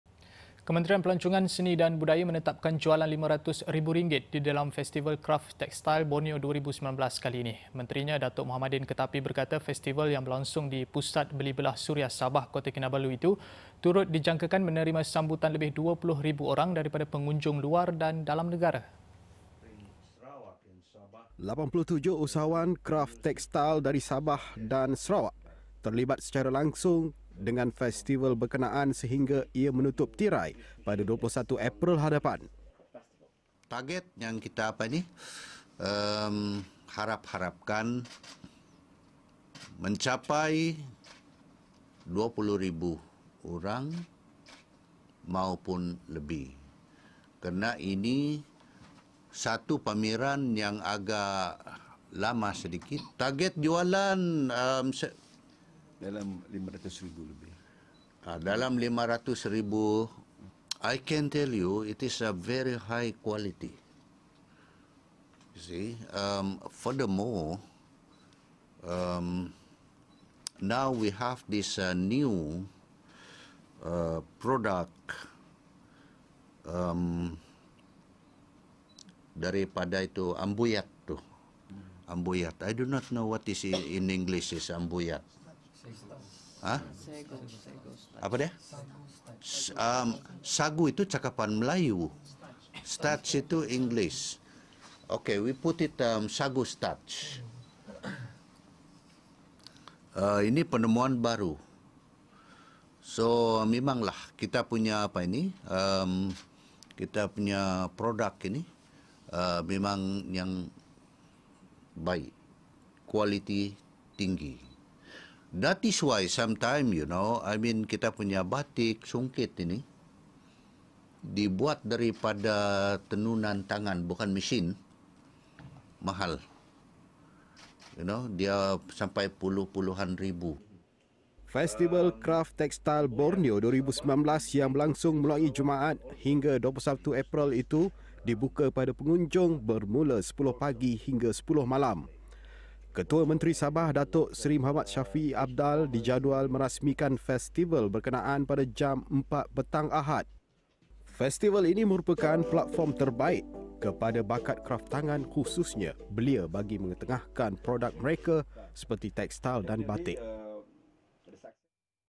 Festival Kraf Tekstil Borneo 2019 sasar jualan RM500,000
Kementerian Pelancongan, Seni dan Budaya menetapkan jualan RM500 ribu di dalam Festival Kraf Tekstil Borneo 2019 kali ini.